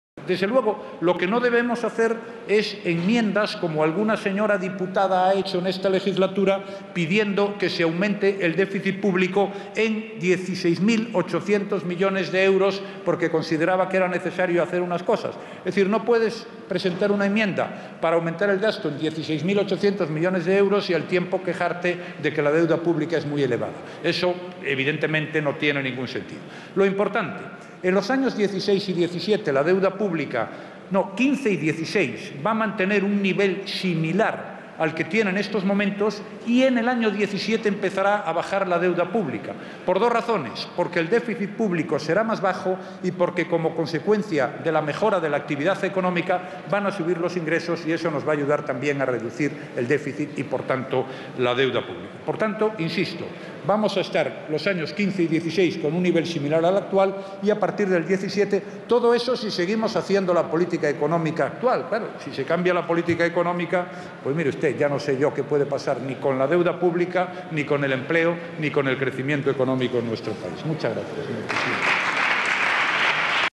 La deuda pública empezará a bajar en 2017
El presidente del Gobierno ha corroborado este miércoles que la deuda pública se mantendrá en su entorno actual durante los años 2015 y 2016, y que comenzará a bajar a partir de 2017 gracias a la recuperación económica y a la contención del déficit público, siempre y cuando "la política económica no cambie" de rumbo.